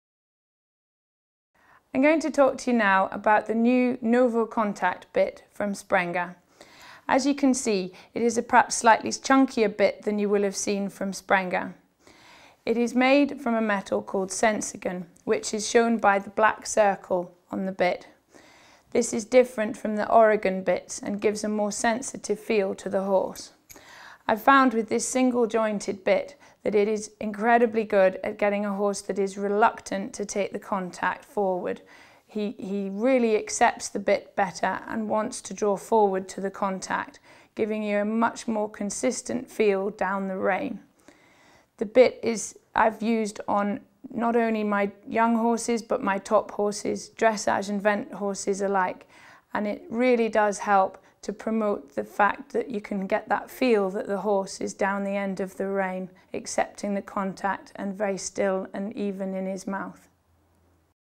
The NovoContact Single Jointed snaffle
The Herm Sprenger NovoContact Single-Jointed Loose Ring Bit is ideal for the horse that is reluctant to take contact from the bit or does not take contact confidently, and encourages the horse to move forward. As the rider gives a rein aid, the mouthpiece turns forward and the contact surface gets narrower, ensuring more precise communication.